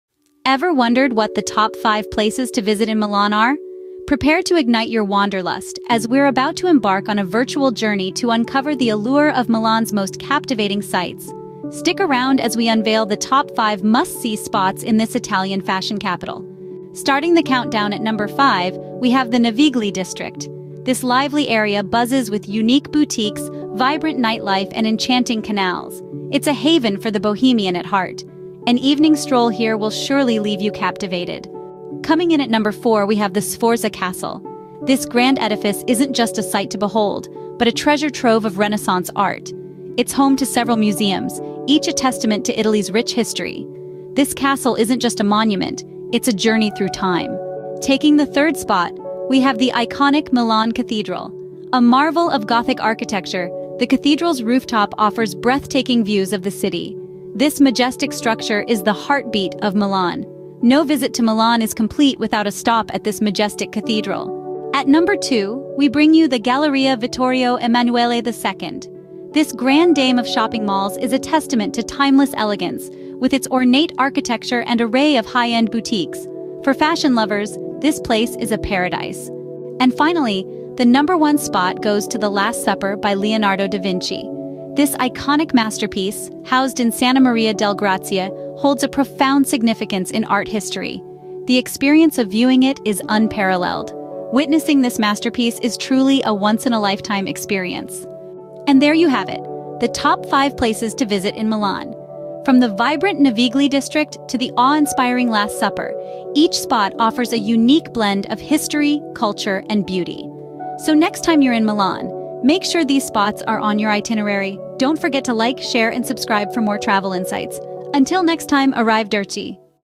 🇮🇹 Explore the Charms of Milan 🇮🇹 | A Journey through Culture, Cuisine, and City Life!
Join us on an exciting adventure through the heart of Milan, Italy! 🏰🍝 Immerse yourself in the rich cultural tapestry, savor the exquisite cuisine, and experience the vibrant city life. From iconic landmarks to hidden gems, we’re here to unveil the beauty of Milan. 🎥✨